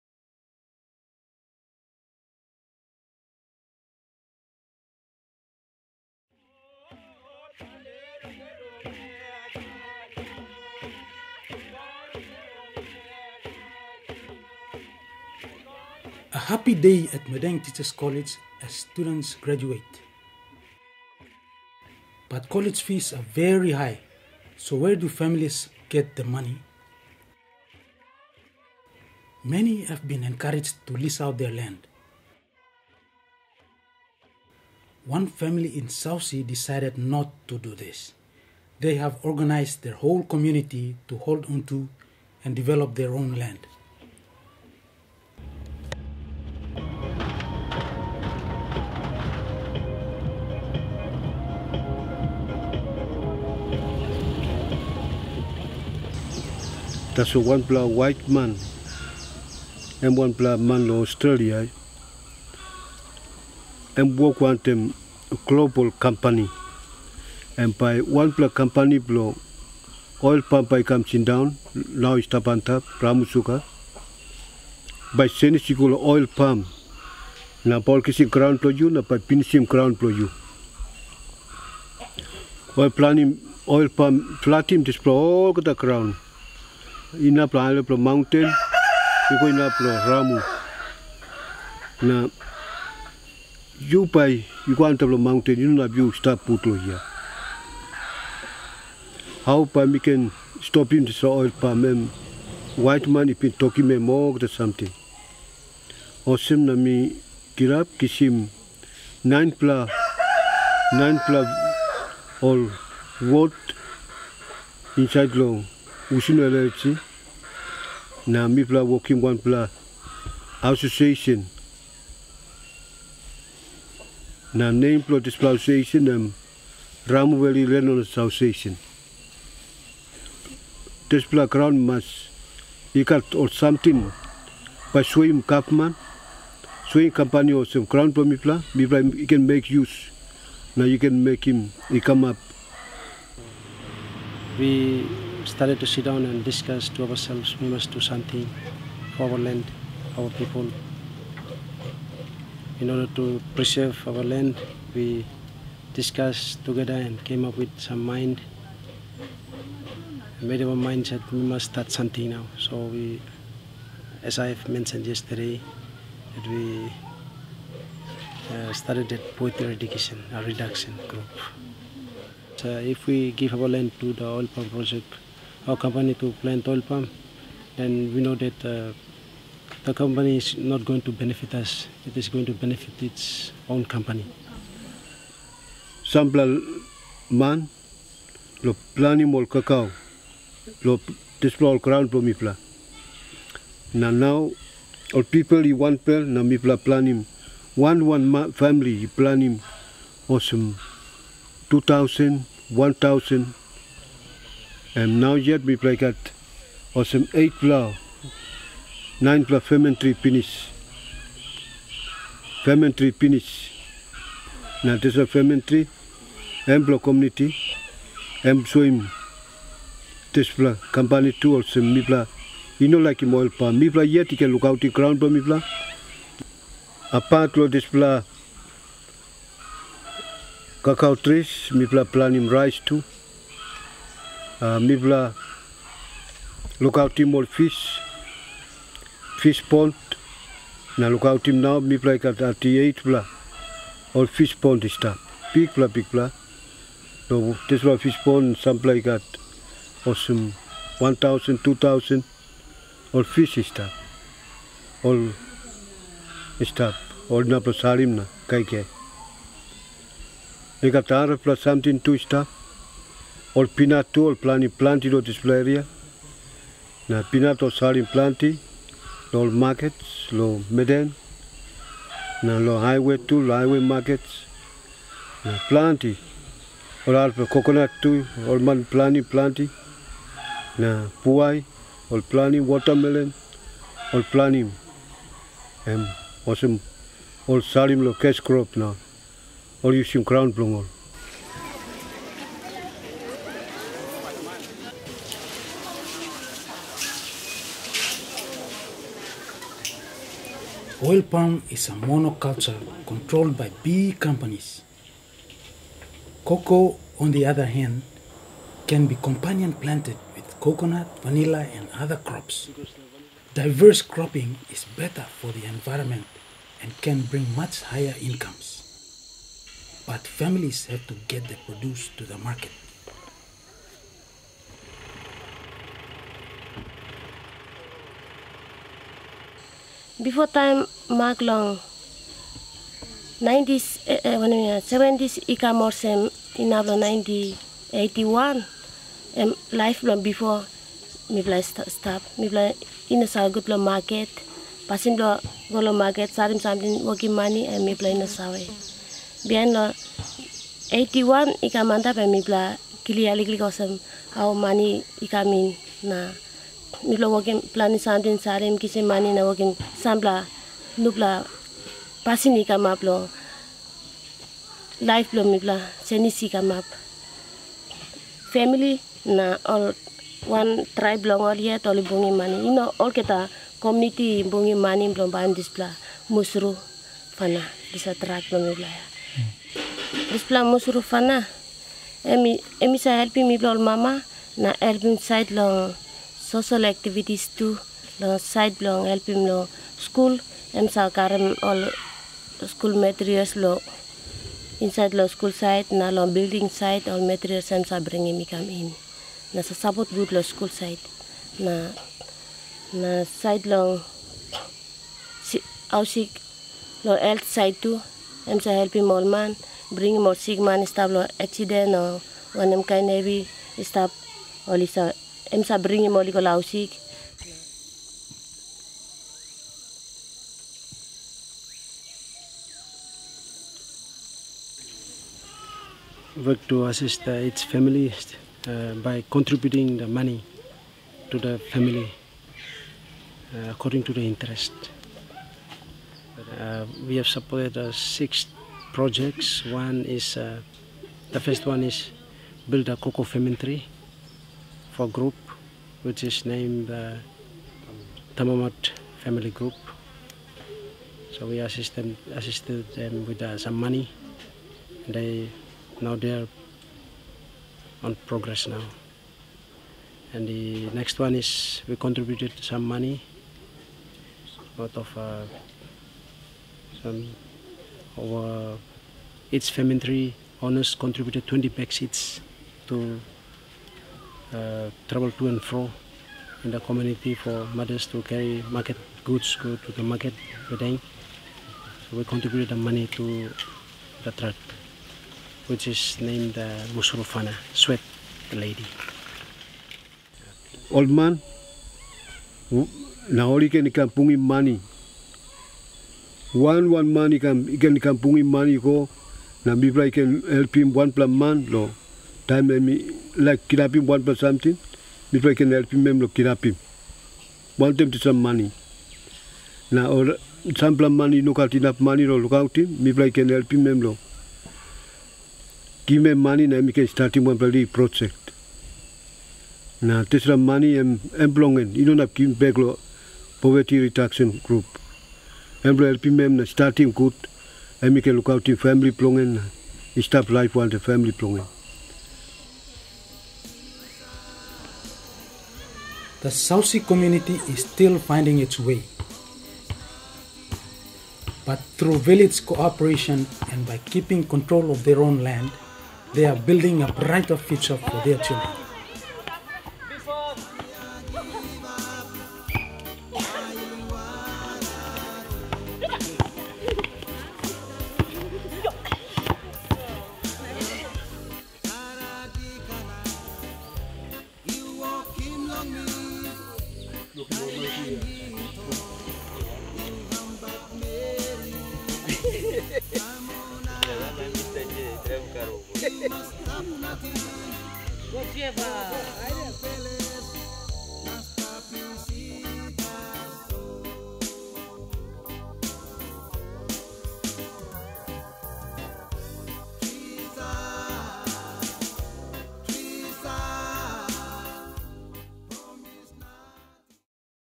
Sausi: village cooperation
Interview with Aipapu Maria, from Sausi in the Upper Ramu (Aparamu, Madang Province, Papaua New Guinea), on their resistamnce to oil palm expnasion and their own forms of village investment and local development